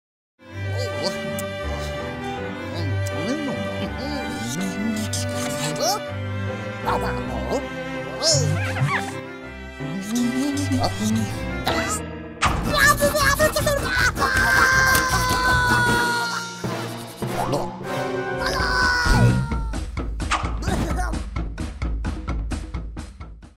MAPED felt pens and 2 Rabbids. What could possibly happen?
The Rabbids are real artists! Watch them paint their masterpiece